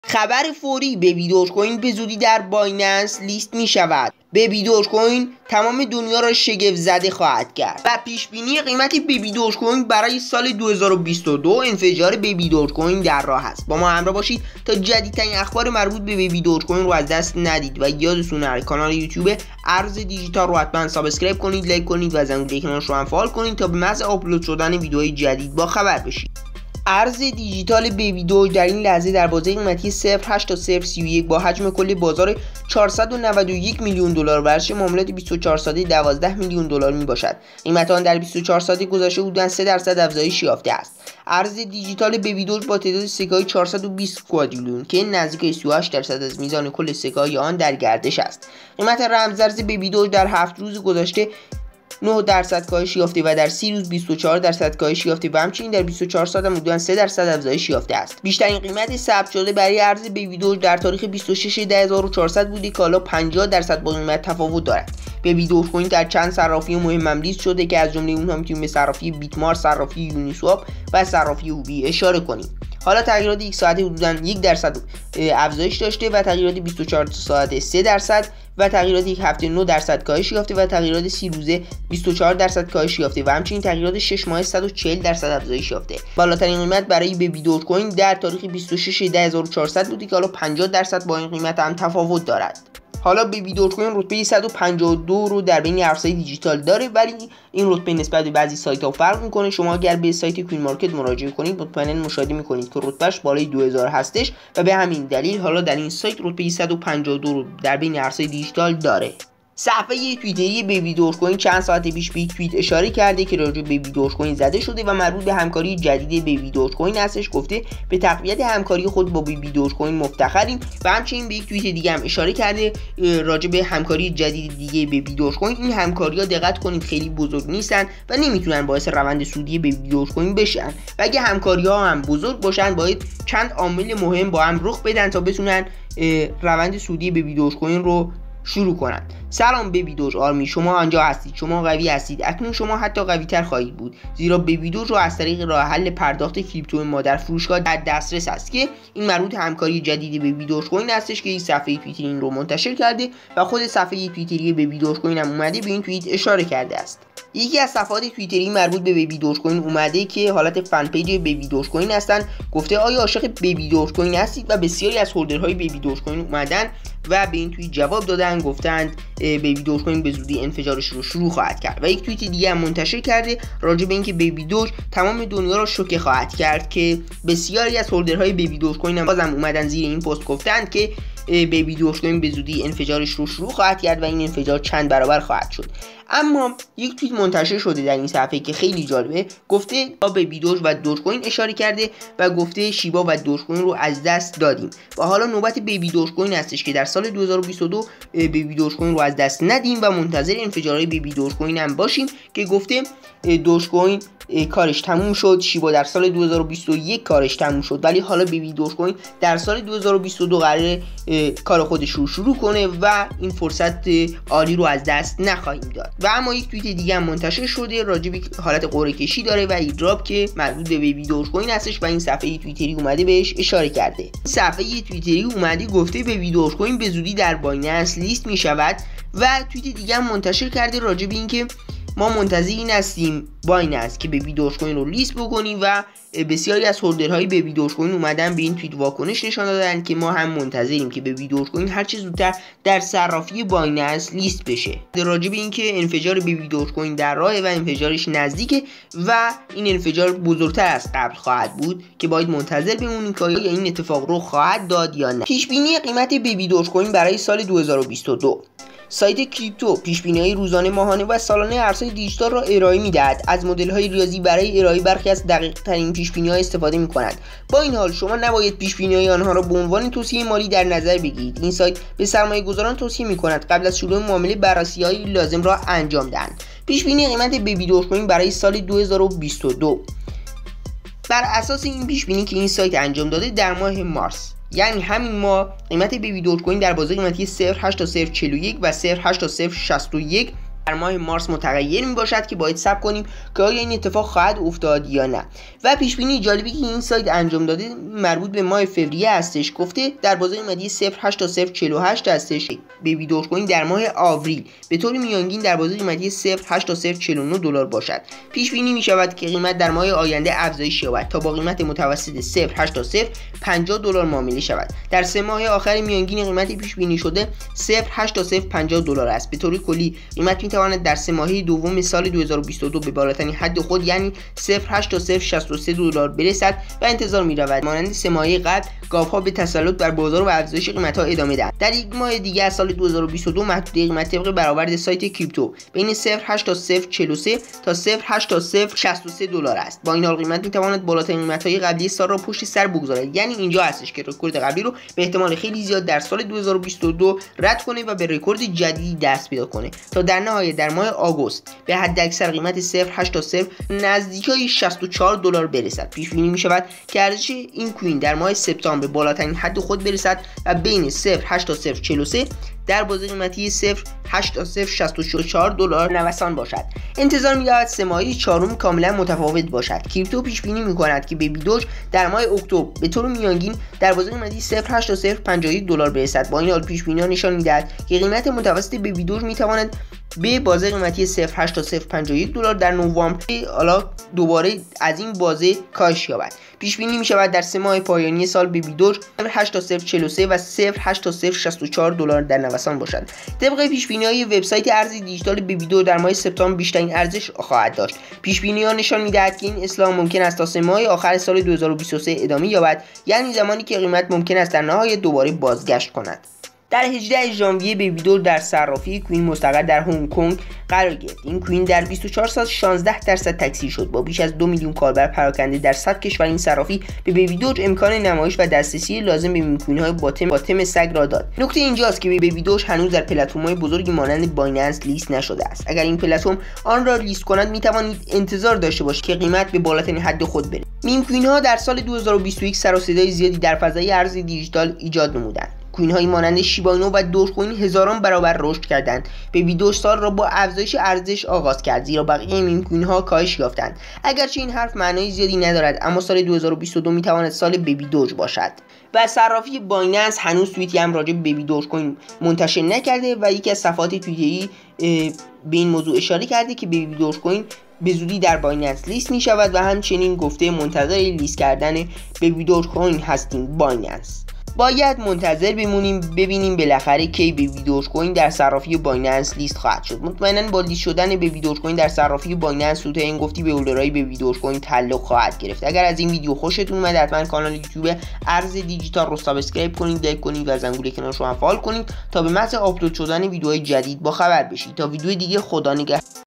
فوری انفجار بیبی دوج شروع شد؟ | خبر مهم بیبی دوج در بایننس لیست می شود؟ | همکاری جدید بیبی دوج کوین؟
سلام در این ویدیو به تحلیل و بررسی بیبی دوج کوین پرداختم انفجار بیبی دوج کوین شروع شد؟ خبر فوری بیبی دوج کوین در بایننس لیست می شود؟ همکاری جدید بیبی دوج کوین؟ با ما همراه باشید.